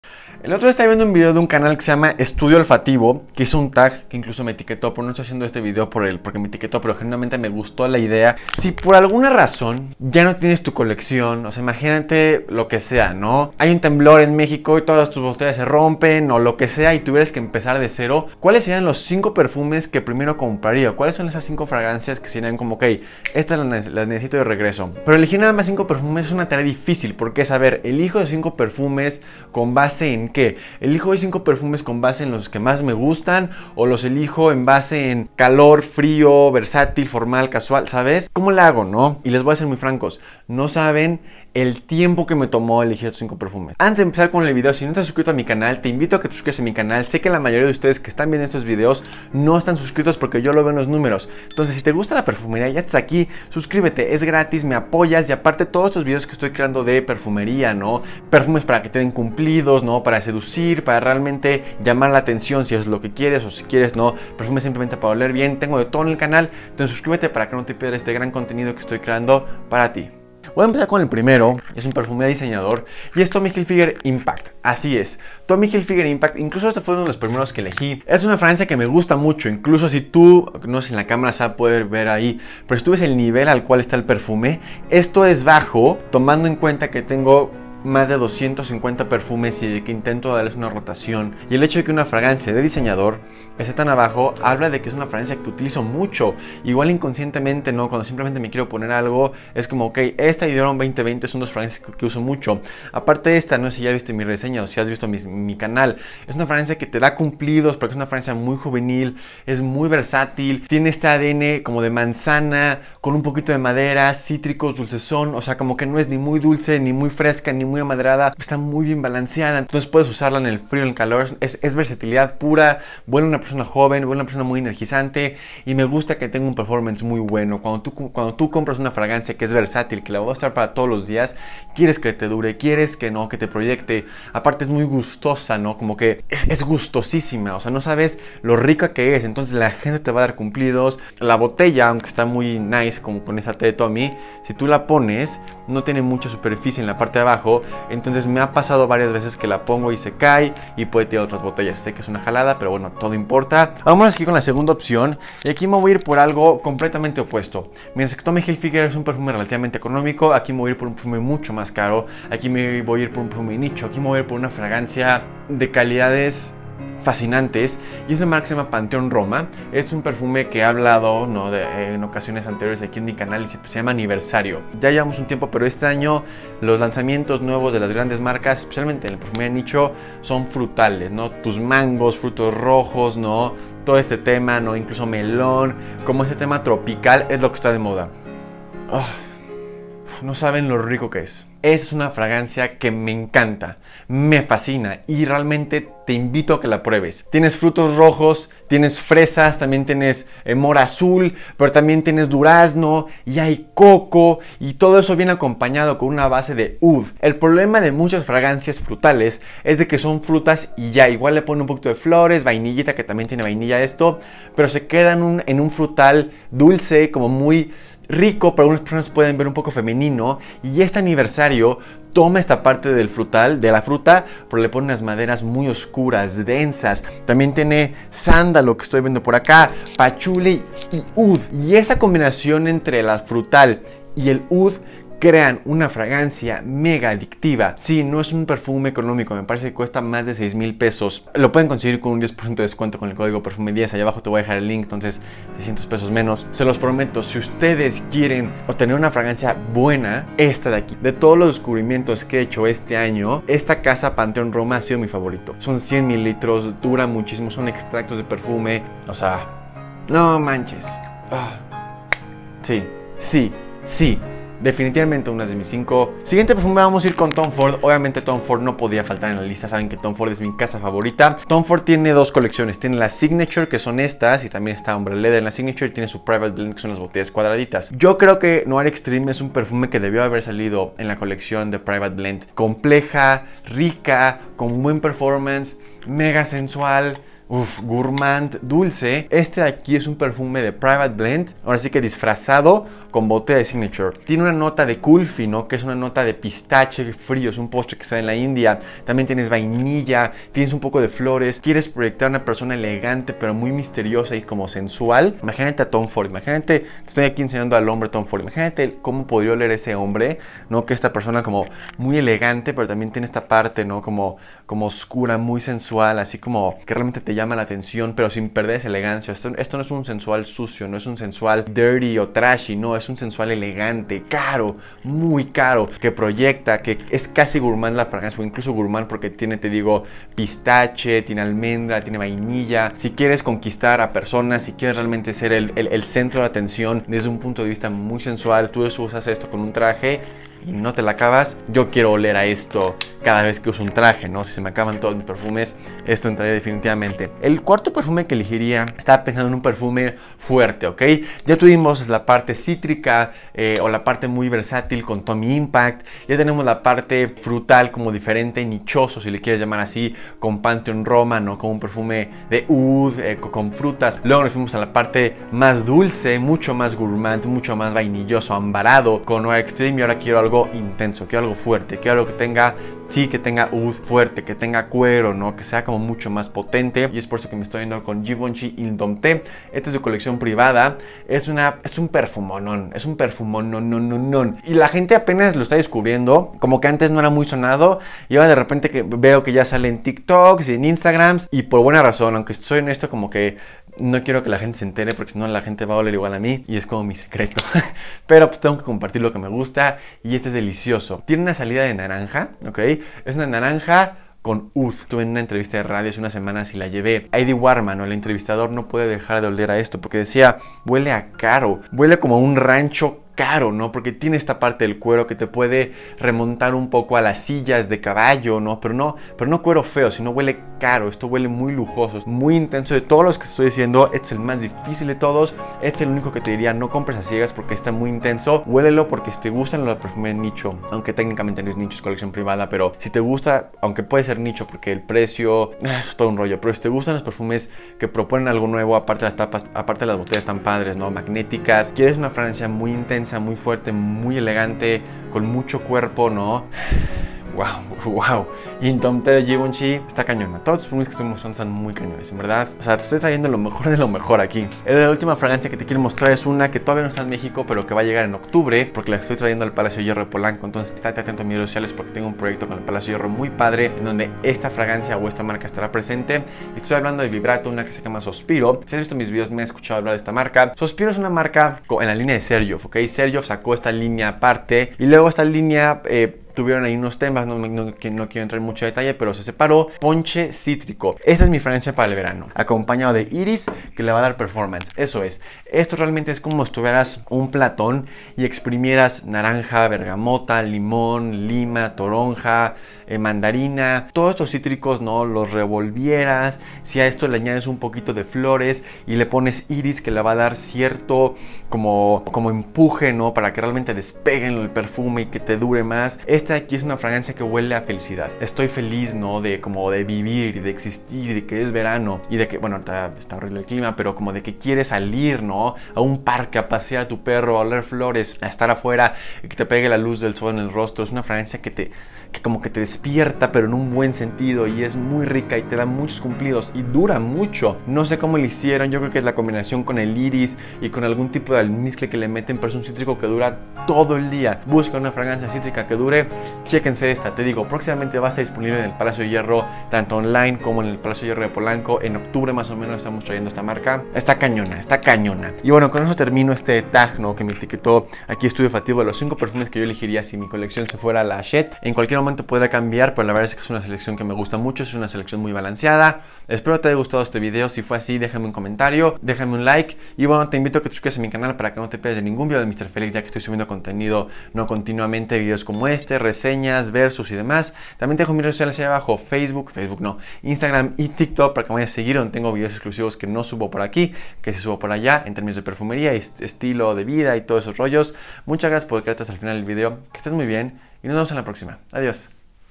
Fragancias Con Las Que Empezaría Mi Colección
Si por alguna extraña razón perdiera toda mi colección, estas serían las primeras 5 fragancias que compraría de nuevo. Gracias a @Estudio-Olfativo por etiquetarme y por la idea.